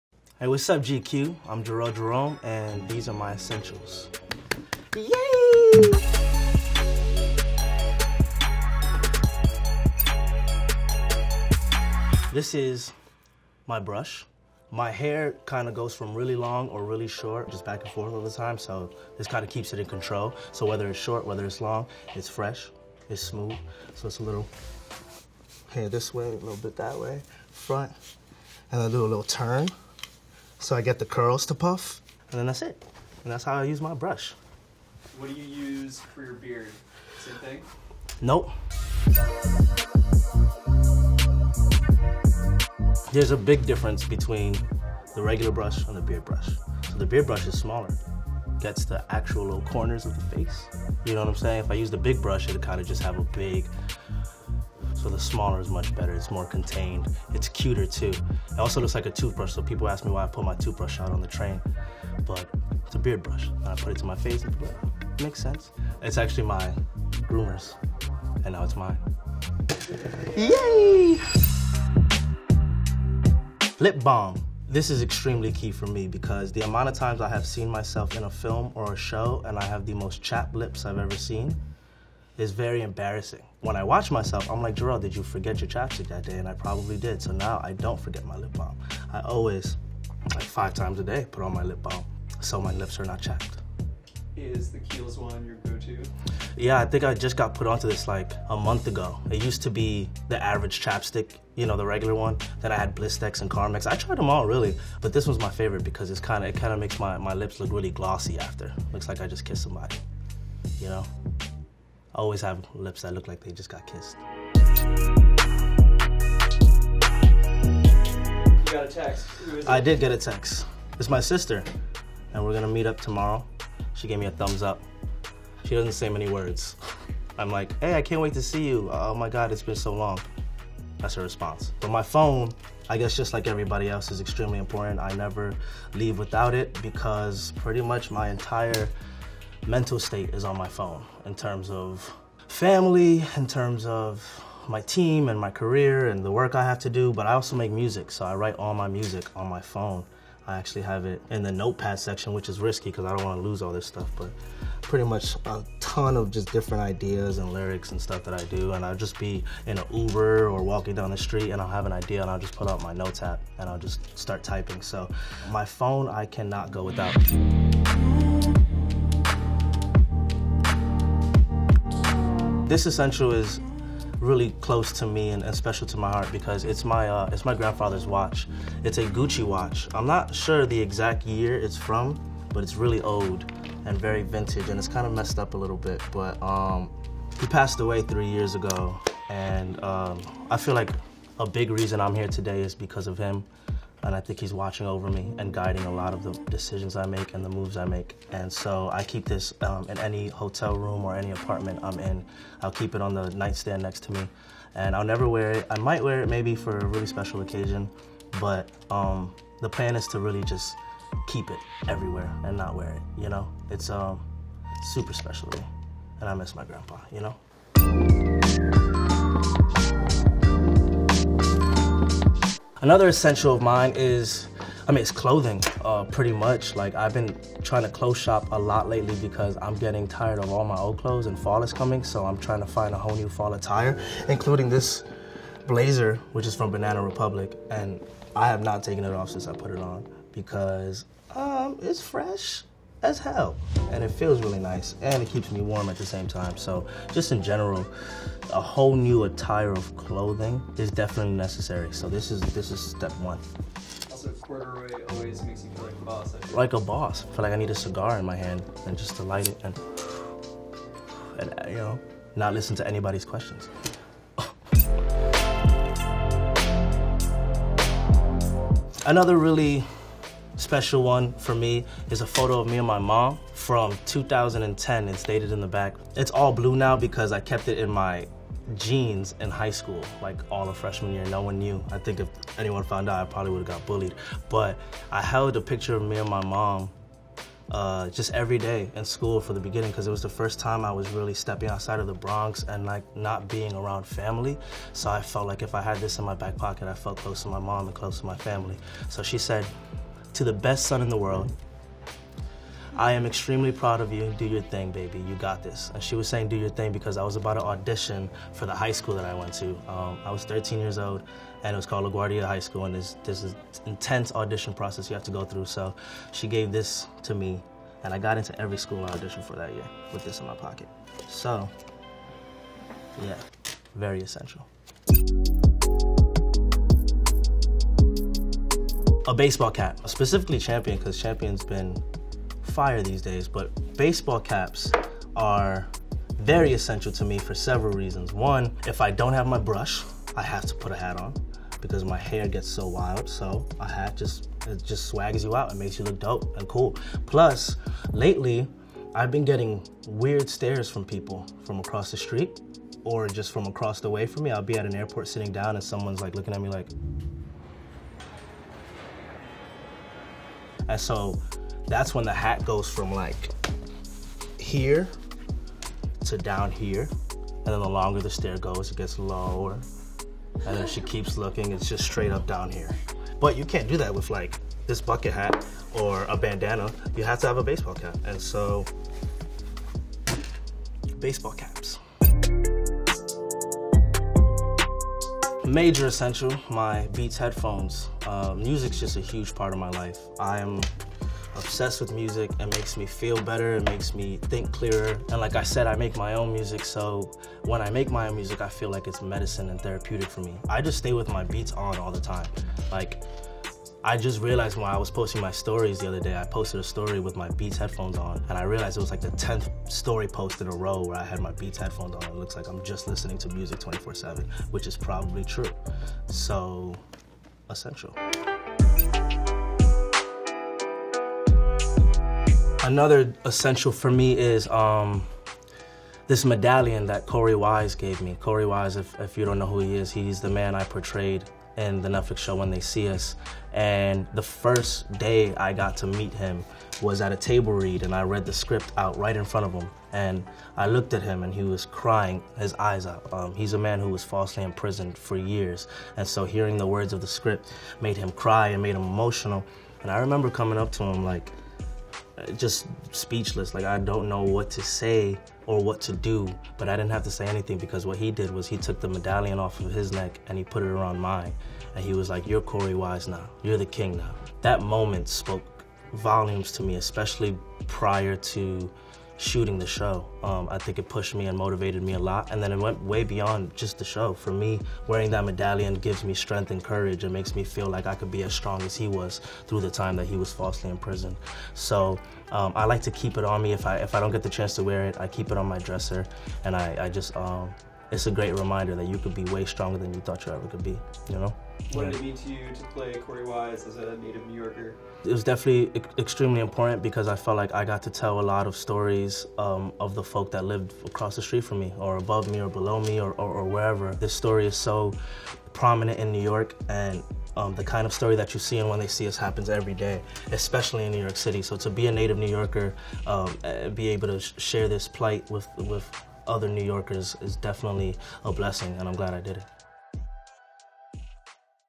10 Things Jharrel Jerome Can't Live Without | GQ
Presented by Banana Republic. There are a few things Jharrel Jerome can't live without when he hits the road. From his hair and beard brushes to his grandfather's vintage Gucci watch, these are Jharrel's travel essentials.

10 Things Jharrel Jerome Can't Live Without | GQ